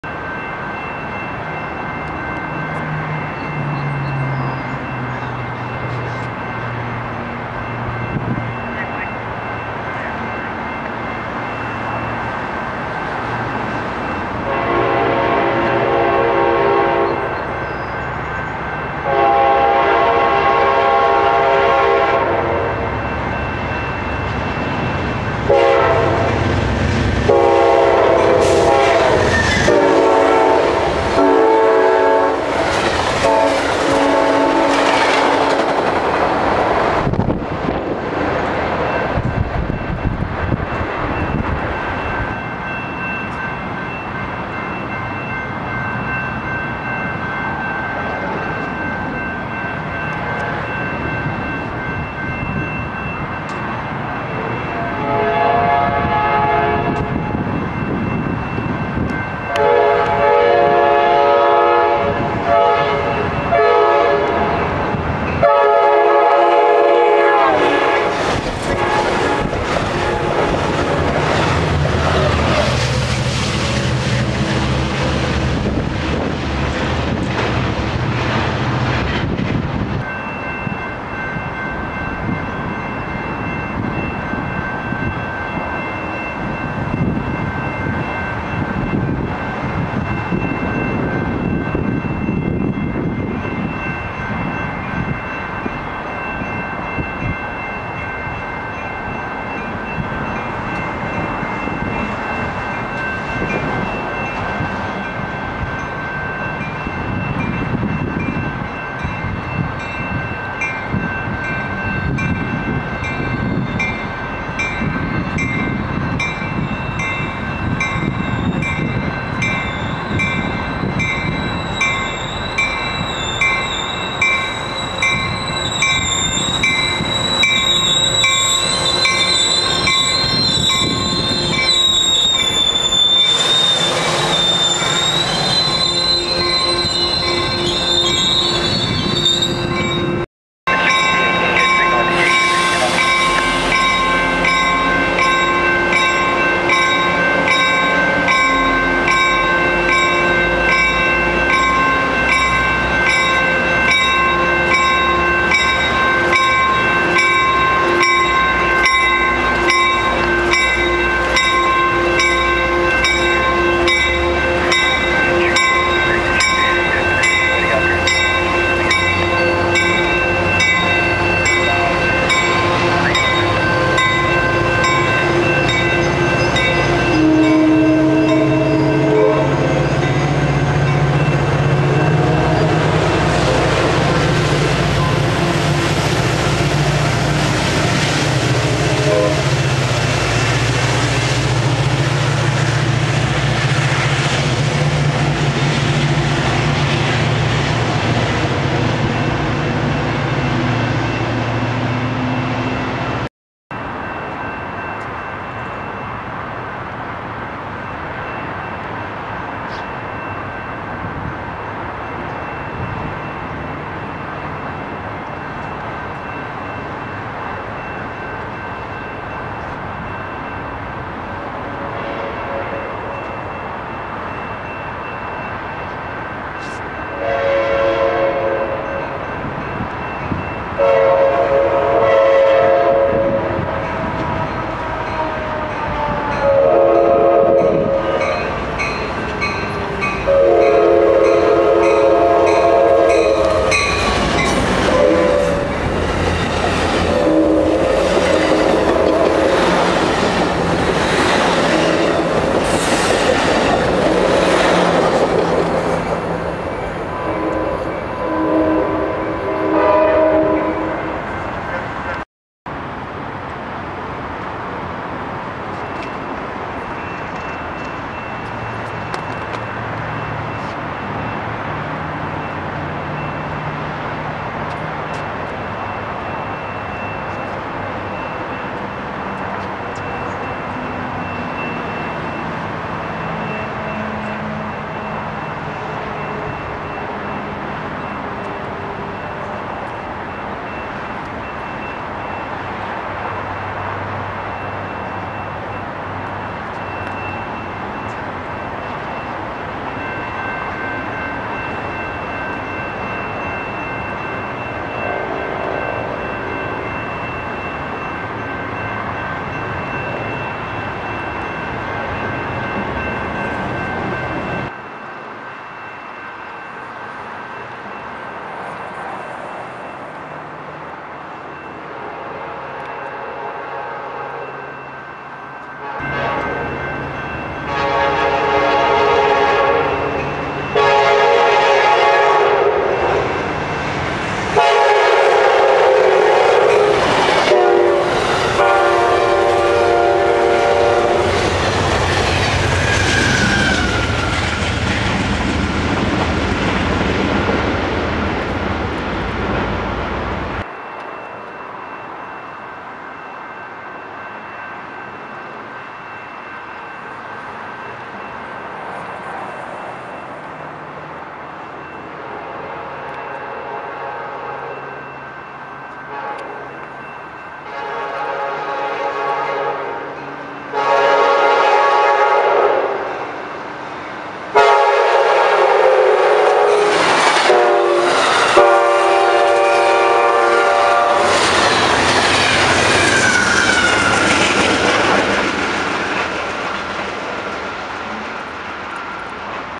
Trains at Sorrento Valley with Amtrak Meet
Went out for some railfanning and caught some good trains. AmtrakCal462 joined me for the first train.